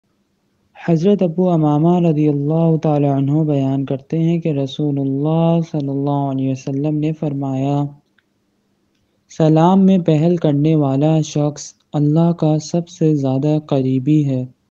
Salam Karne Ke Masail | Salam Ka Tarika | Salam Ke Ahkam
وَعَنْ أَبِي أُمَامَةَ قَالَ: قَالَ رَسُولُ اللَّهِ ﷺ
إِنَّ أَوْلَى النَّاسِ بِاللَّهِ مَنْ بَدَأَ السَّلَام
رَوَاهُ أَحْمد وَالتِّرْمِذِيّ وَأَبُو دَاوُد
ابوامامہ رضی اللہ عنہ بیان کرتے ہیں
رسول اللہ ﷺنے فرمایا
سلام میں پہل کرنے والا شخص
 اللہ کا سب سے زیادہ قریبی ہے
رواہ احمد و الترمذی و ابوداؤد